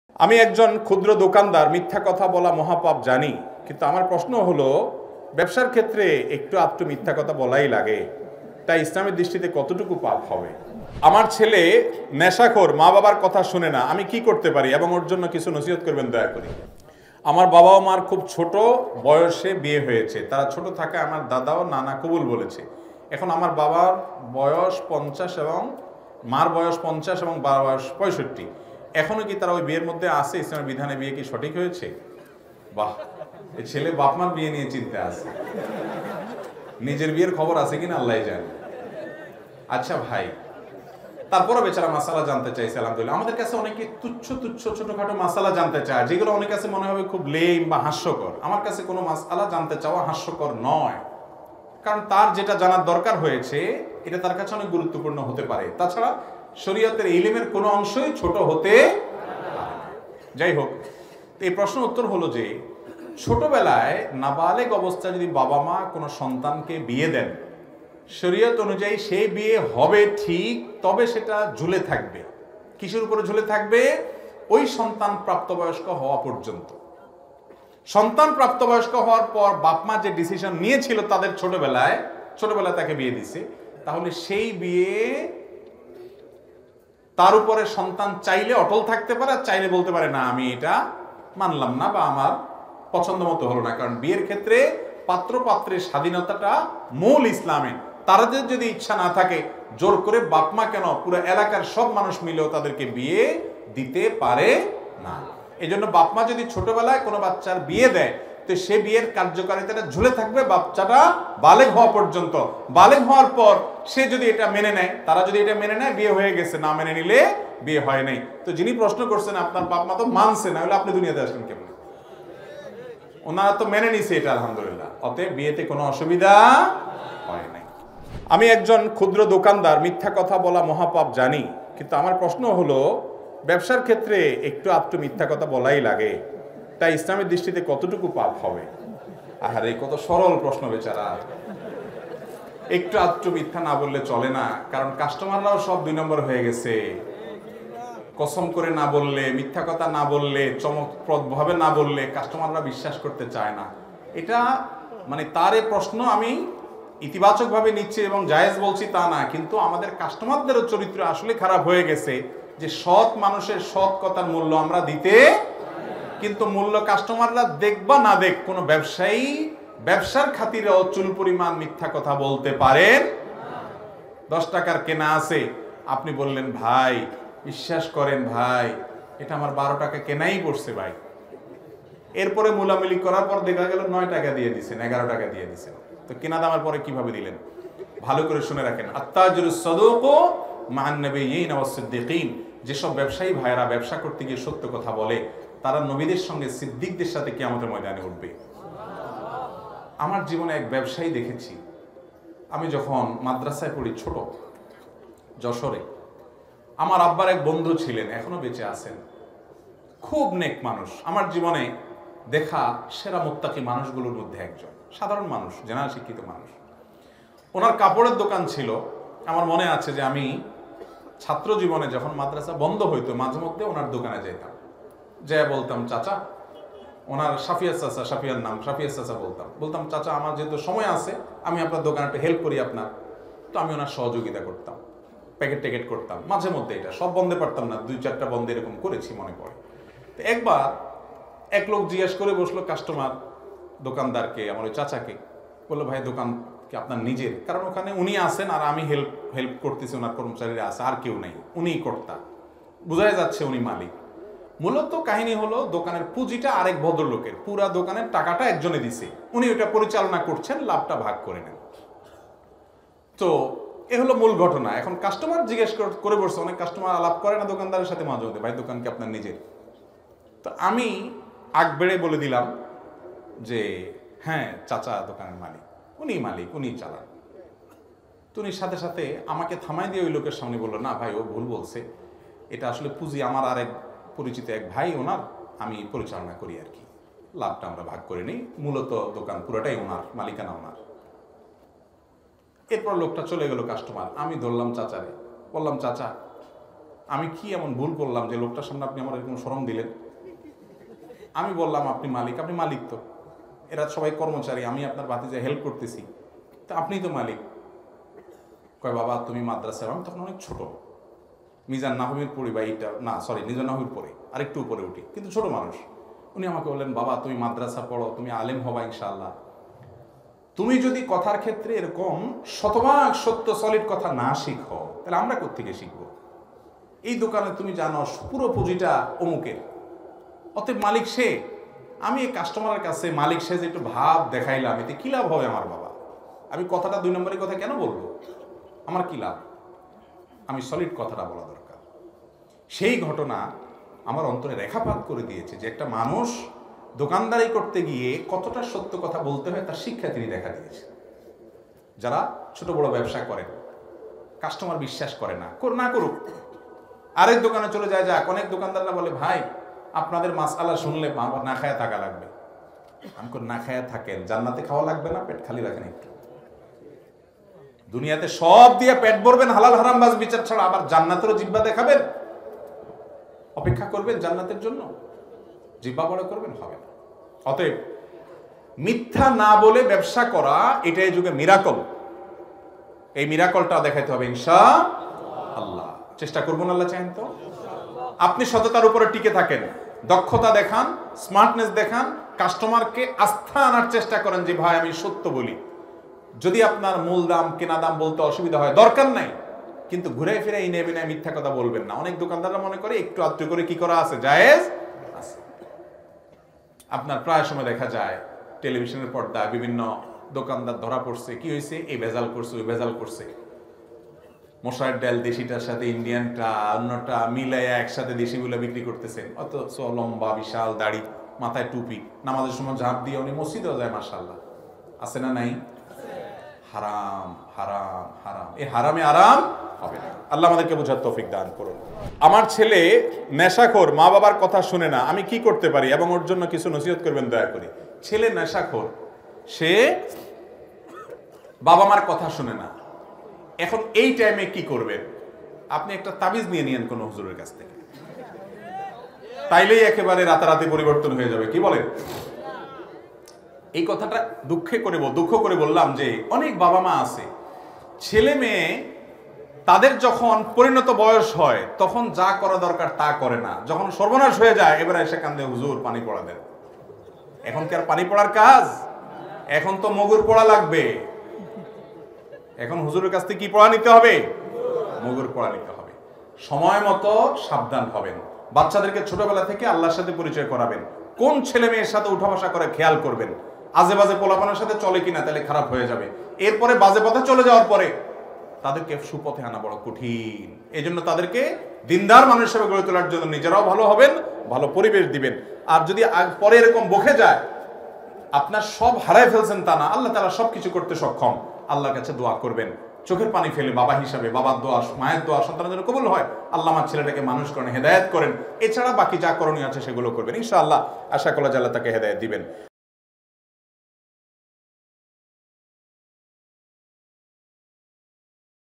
পিতার বিয়ে নিয়ে সন্দেহ প্রকাশ করে যে প্রশ্ন করলেন সন্তান
পিতার বিয়ে নিয়ে সন্দেহ প্রকাশ করে যে প্রশ্ন করলেন সন্তান new waz II waj II Sheikh ahmadullah